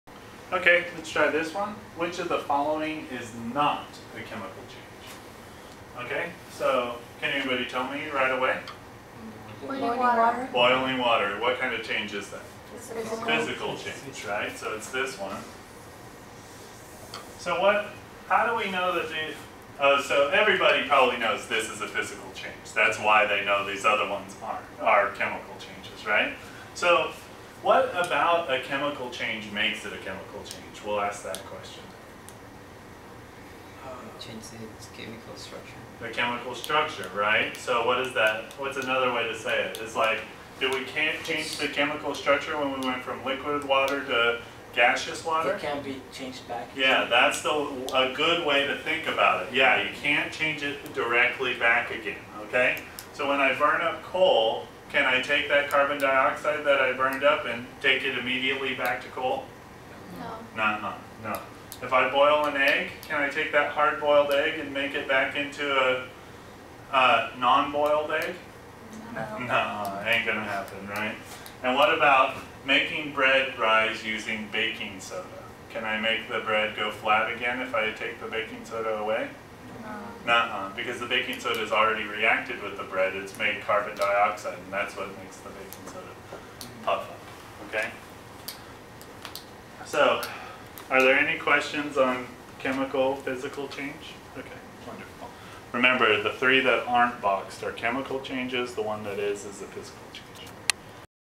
1411 PE01AMC 012 - Identifying Chemical and Physical Changes
Which of the following is not a chemical change?
A. burning coal
B. making bread rise using baking soda
C. boiling an egg
D. boiling water